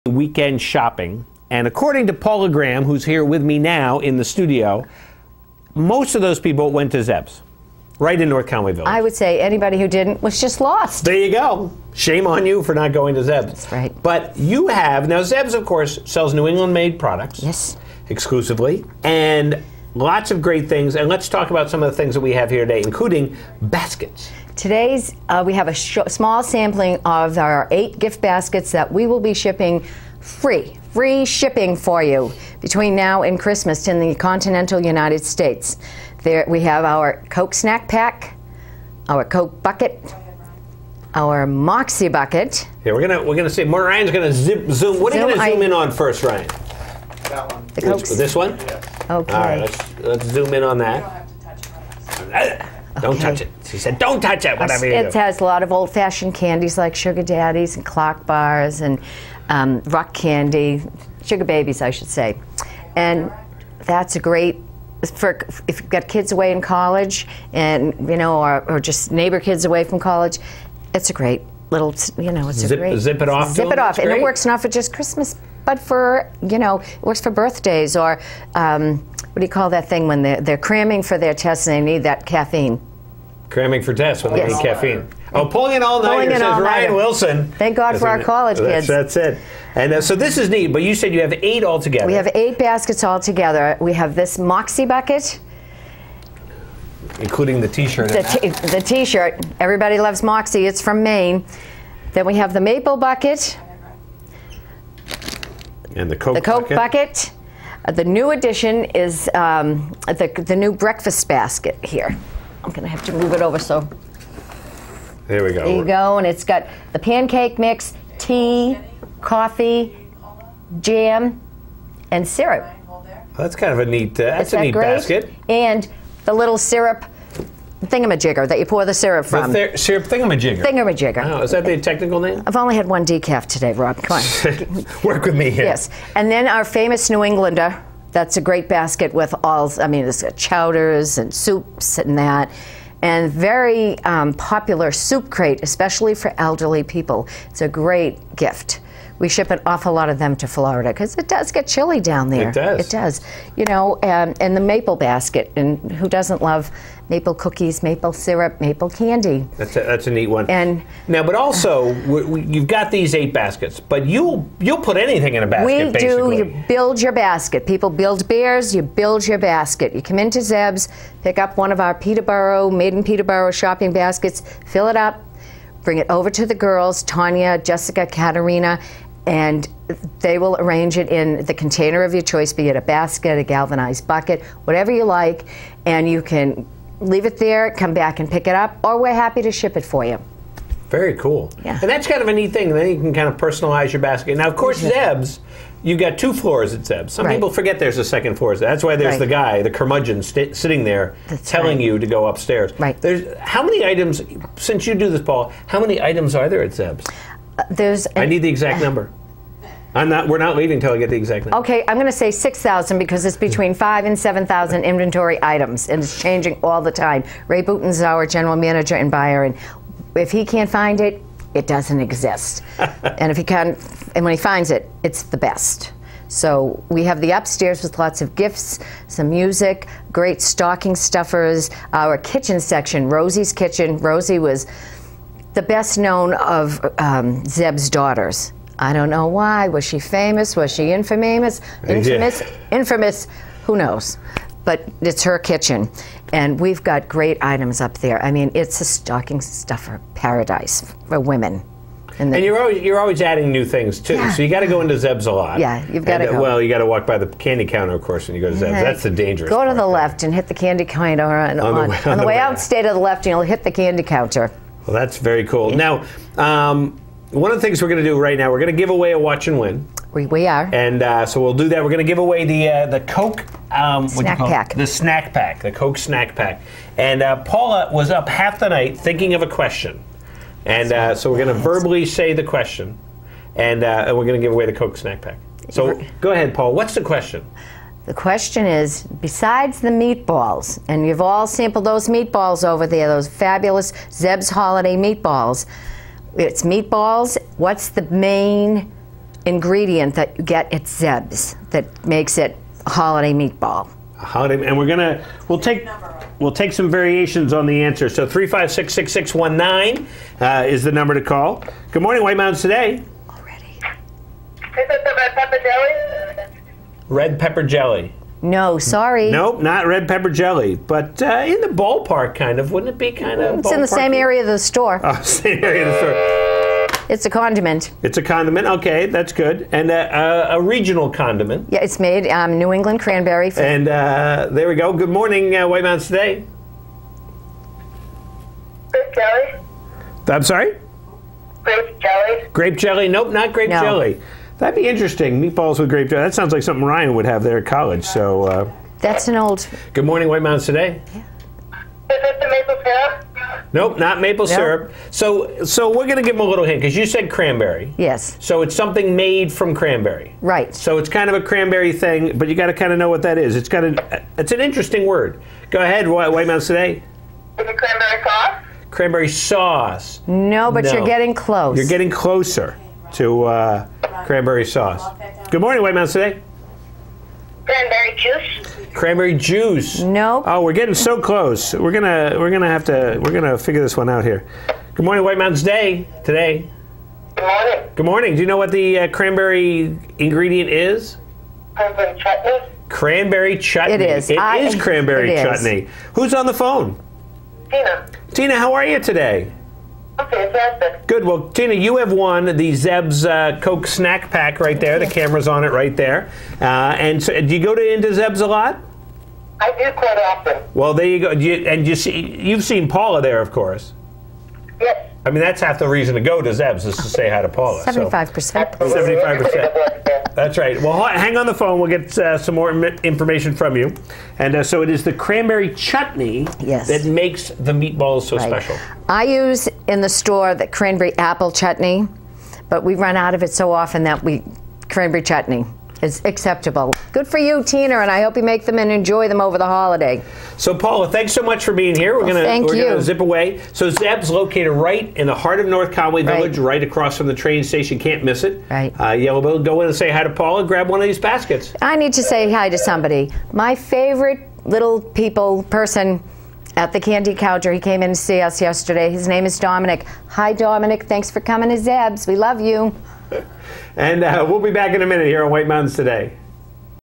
Zeb's Country Store
Paula Graham talks about just some, of the thousands of great holiday gift ideas at Zeb's Country Store.